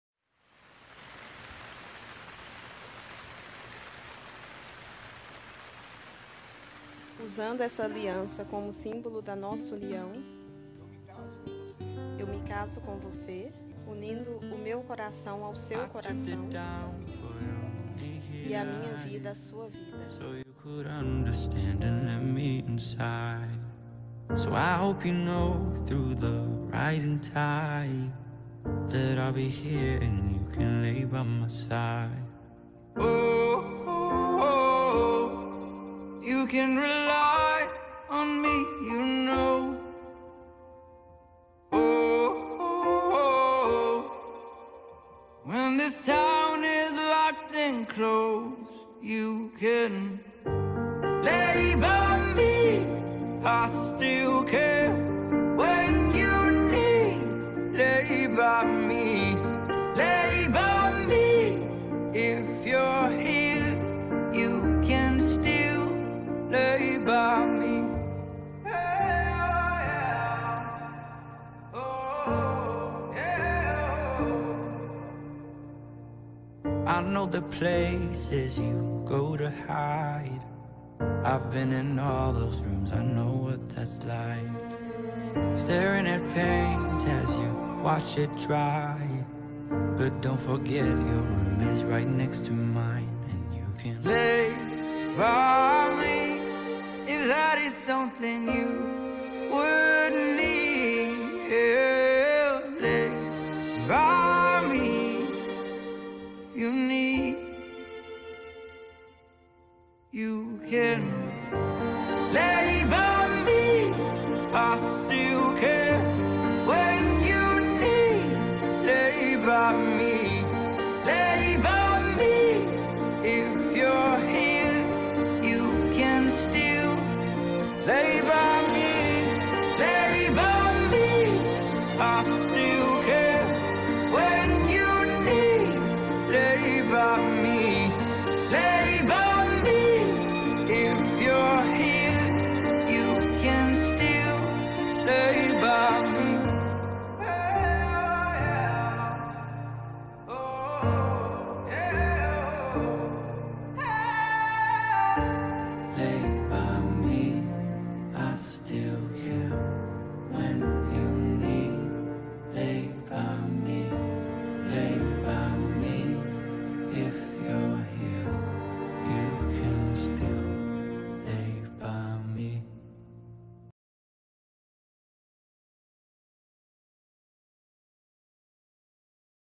EMOCIONANTE!!! Não achamos outra palavra para expressar esse lindo vídeo de Casamento na Pampulha
Convidamos a assistir esse lindo vídeo de casamento na Pampulha em BH no Colonial Pampulha.
Um vídeo carregado de Emoções e Inspirações...